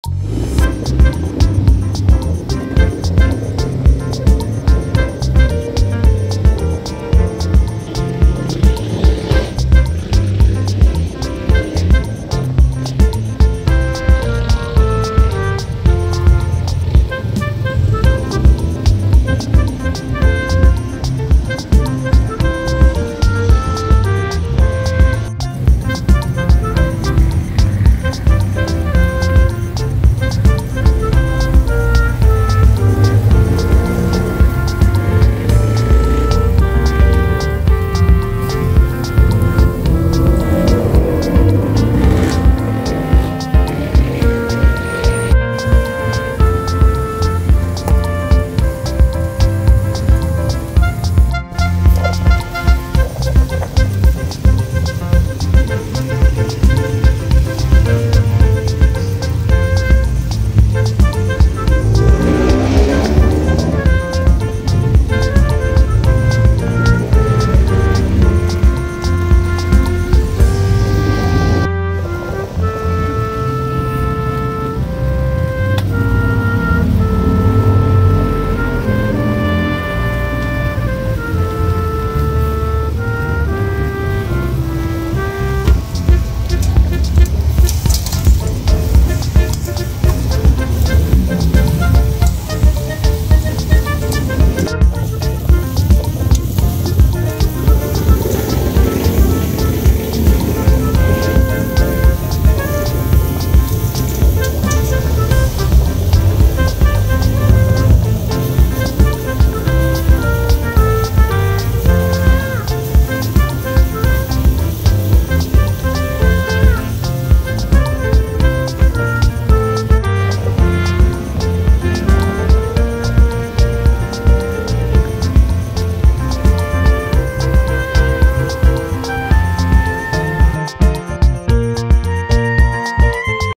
GRILLED PORK BBQ and CHICKEN INASAL | KaBatang na IBANAG.
KaBatang na IBANAG Cooking Mukbang ASMR adventure explore singing and family gathering.
Mag ihaw Naman tayo ng pork BBQ at CHICKEN inasal.masarap na sulit sa lasa.